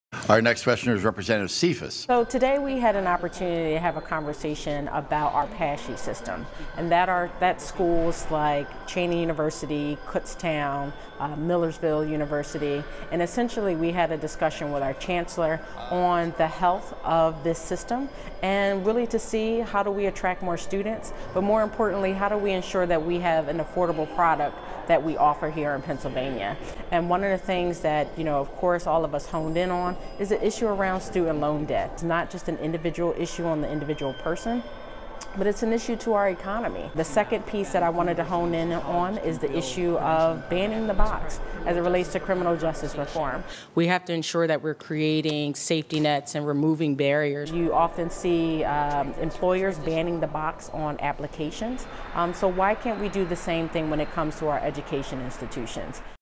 Cephas Fights To Make Higher Education Affordable, Accessible
Pa. state Rep. Morgan Cephas is fighting to make higher education affordable and accessible for everyone. She talked with Pennsylvania’s State System of Higher Education about reducing student loan debt and the need to "ban the box" on college applications.